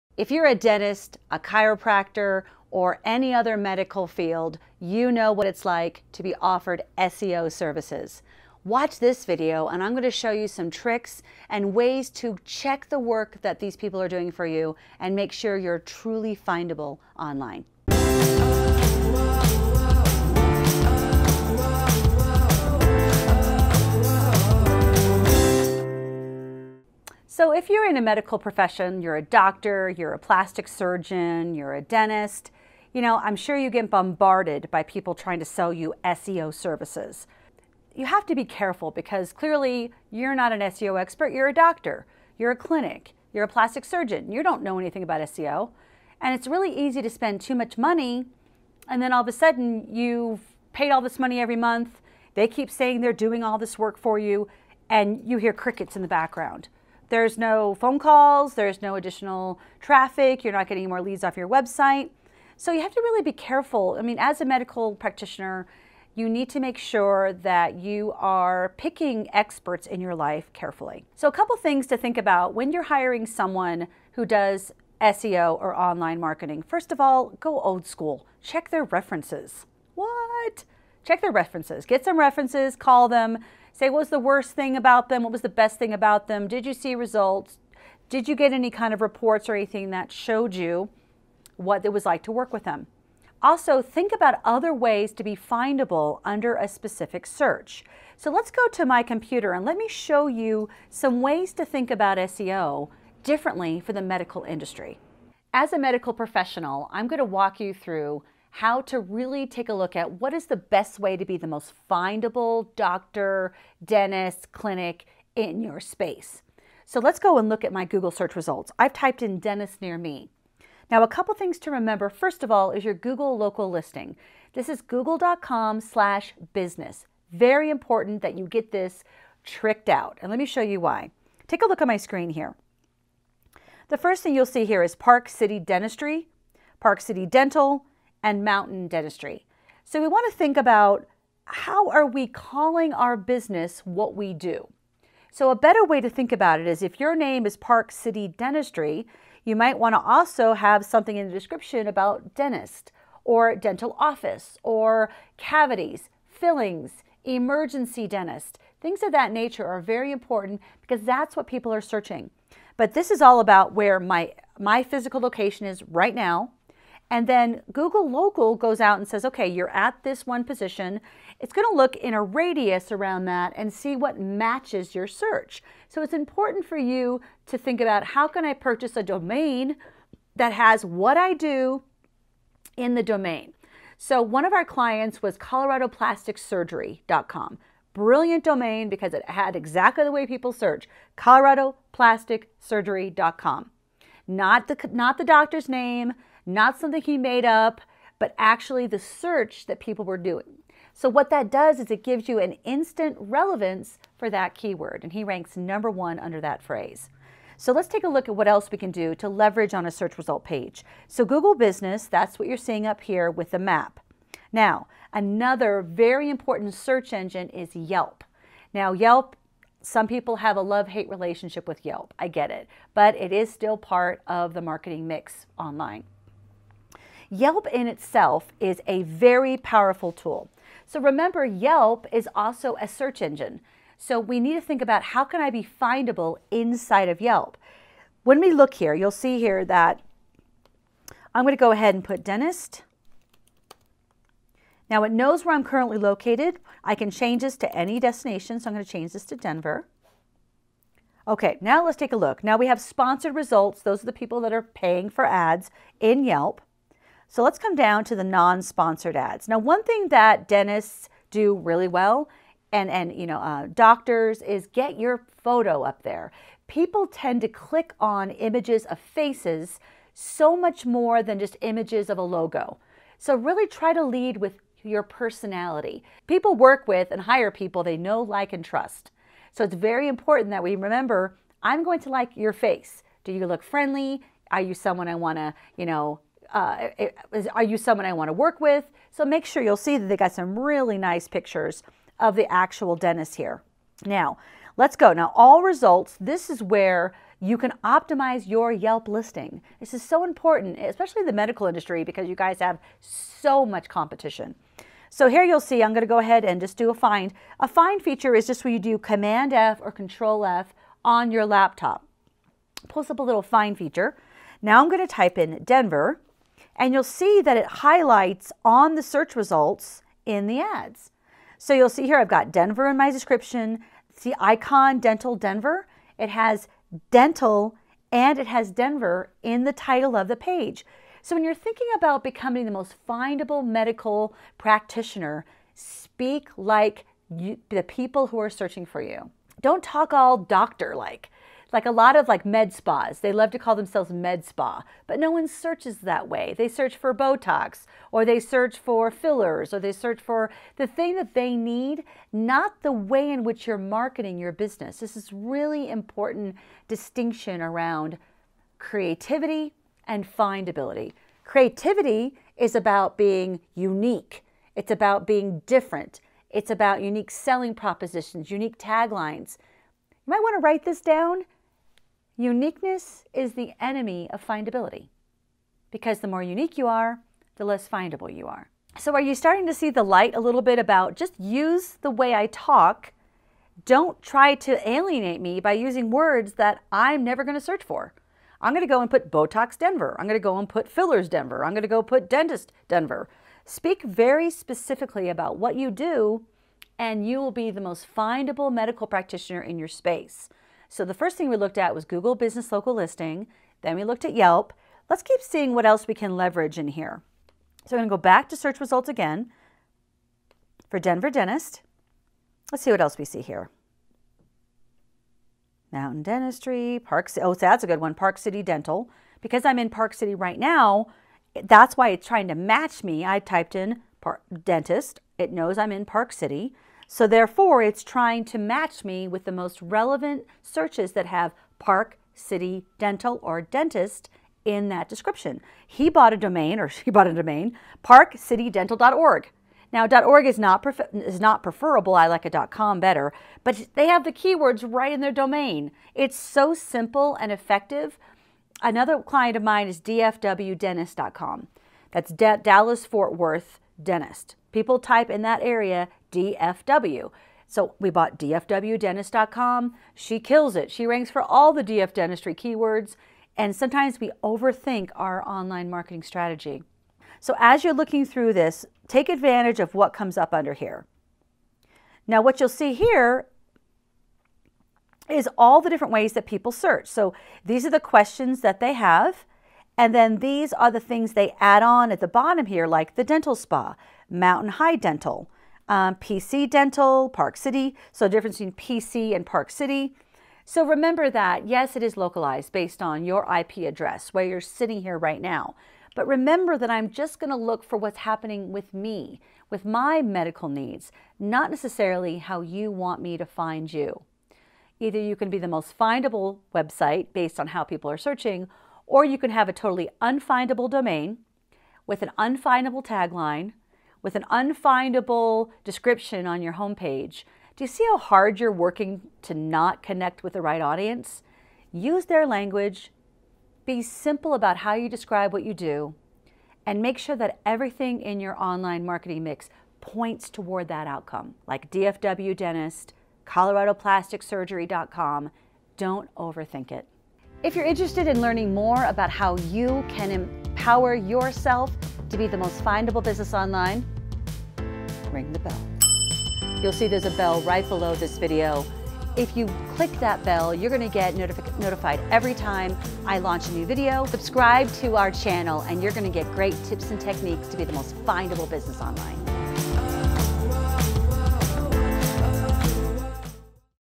Medical SEO
Are you a medical practitioner? Have you been contacted by so-called SEO professionals that offer you SEO services? Have you been duped by any of them? We all know how it feels to avail of services to find that we're not getting any results. And simply put, SEO wasn't made to be understood by everybody, but I can show you the ropes.

I started this company, Findability University, to help people understand what SEO is and what it takes to be findable online. In today's episode, I'm going to go over some basic concepts on what it takes to SEO your medical website and make you and your business findable online. Check it out!

00:27 Are you in the medical profession?
01:12 Go old school - check their references
02:02 Google Local Listing
03:34 Yelp
05:35 Speak like the people who are searching for you
06:10 Uniqueness is the enemy of findability

Heather Lutze

R U N Y O U R W E B S I T E S C O R E

Video by Nate Woodbury
BeTheHeroStudios.com